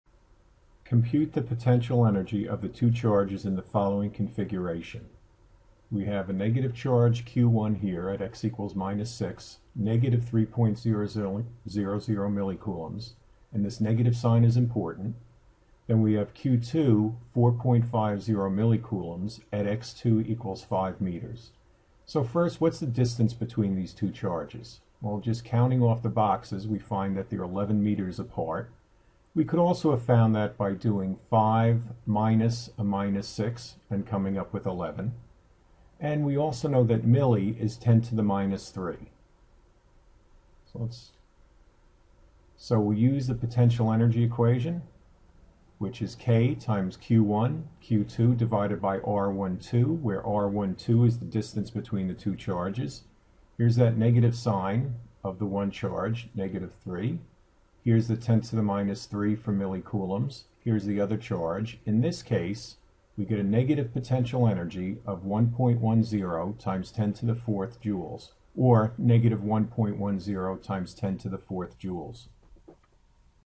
E-field, PE, & Volt. - Electric Potential Energy - Q13
Compute the potential energy of the two charges in the following configuration:

(see video for image)

A negative charge, Q1 = -3.00 mC is located at x1 = -6.00 m, and a positive charge Q2 = 4.50 mC is located at x2 = 5.00 m.